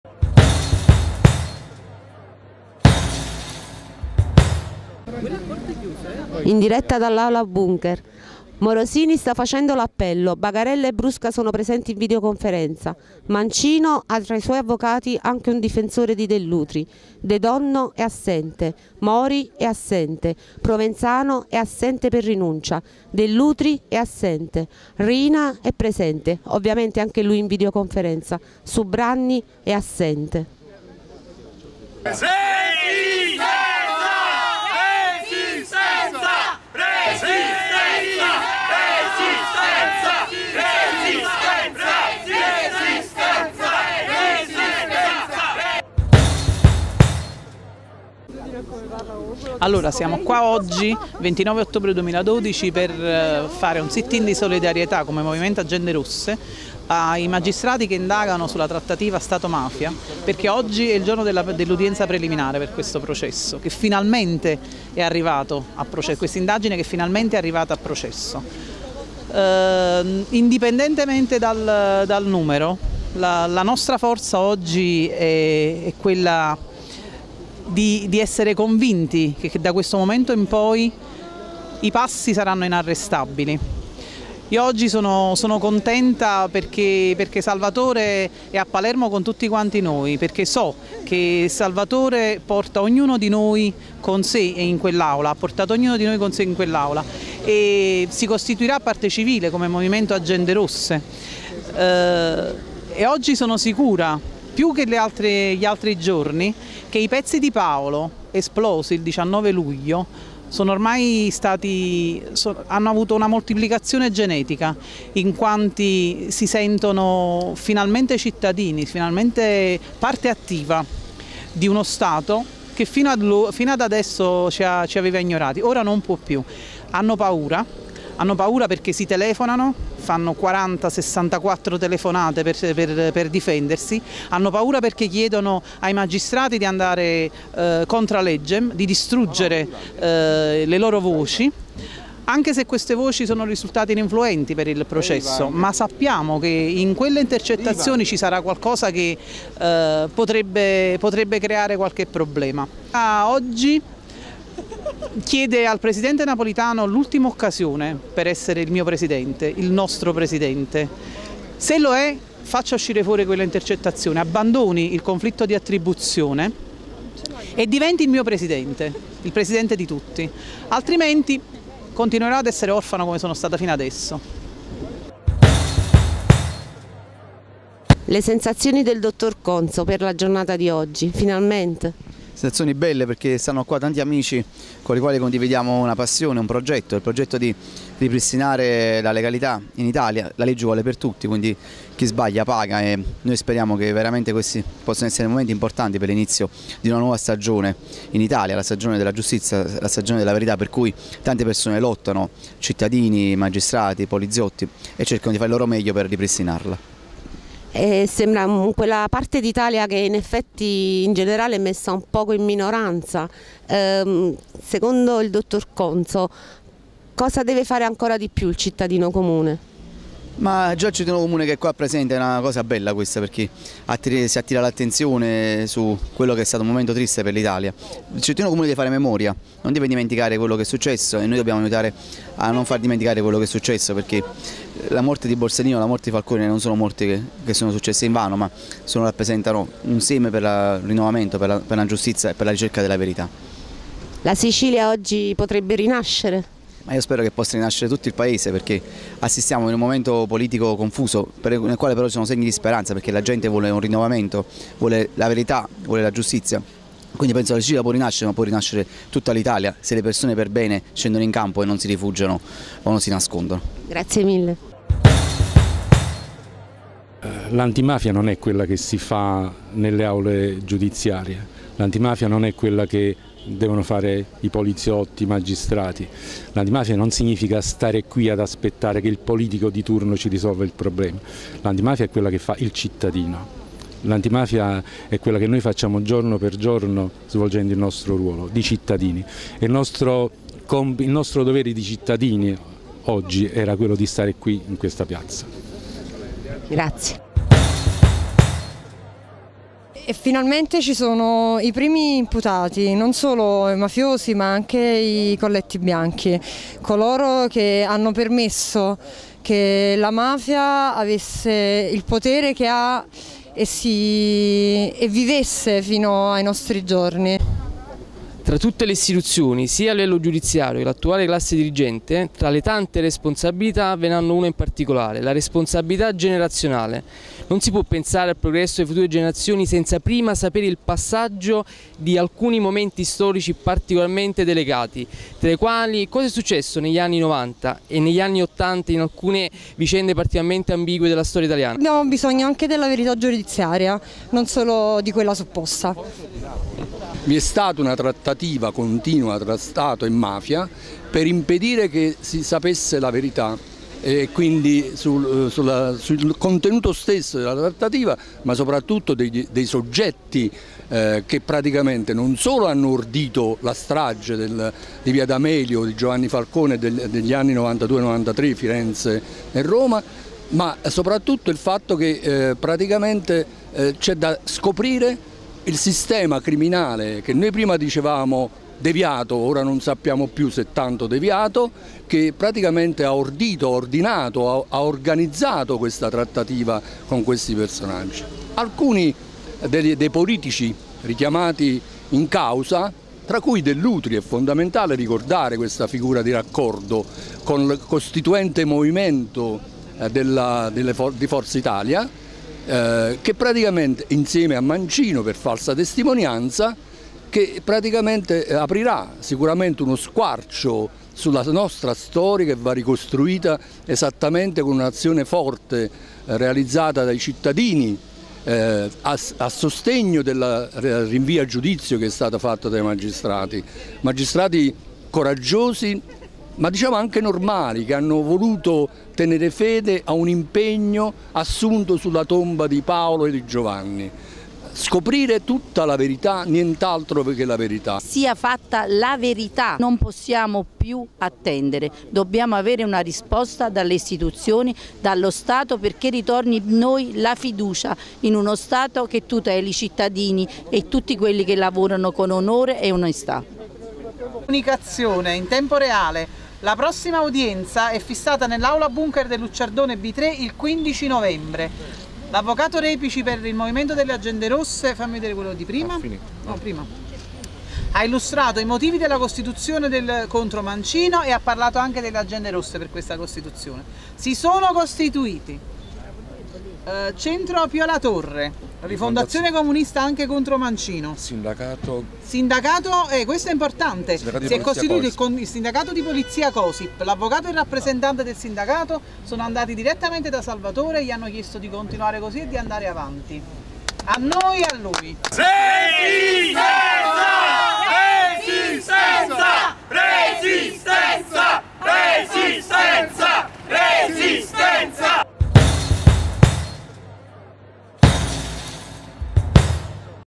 Processo trattativa Stato Mafia - Sit in Napoli 29 ottobre 2012
"Il 29 ottobre a Palermo c'è stata la prima udienza del processo sulla trattativa Stato-Mafia, istruito dalla Procura di Palermo.
Il Movimento Agende Rosse, insieme ad le altre associazioni hanno organizzato un sit-in per sostenere quei magistrati della procura di Palermo che, a causa delle indagini che stanno portando avanti con determinazione, sono sottoposti ad un fuoco concentrico di sbarramento e ad attacchi anche dai gradi più alti delle
Istituzioni.